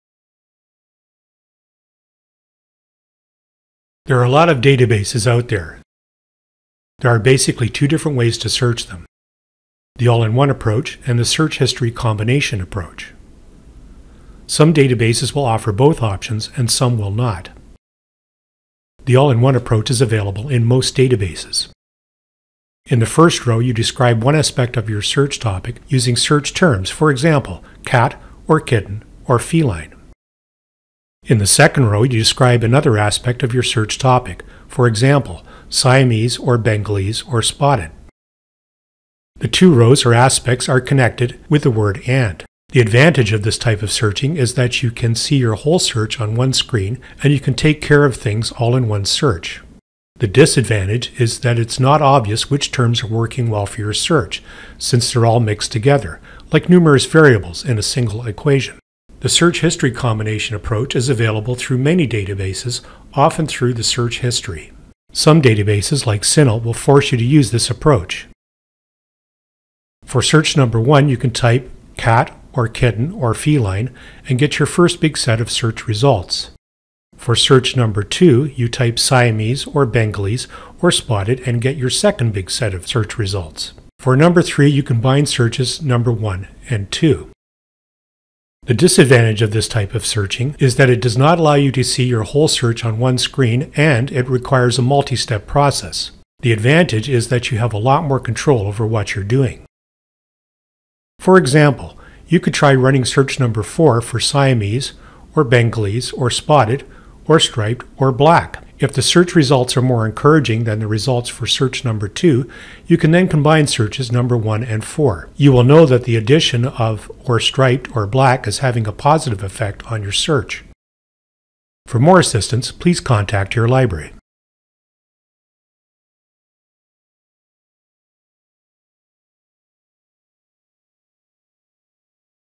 Two Ways To Search a Database
The content of this video may be outdated. For new and improved tutorial content, please visit Western Libraries' Pressbook at the following

A tutorial describing two ways of searching databases: the all-in-one approach, and combining searches using search history.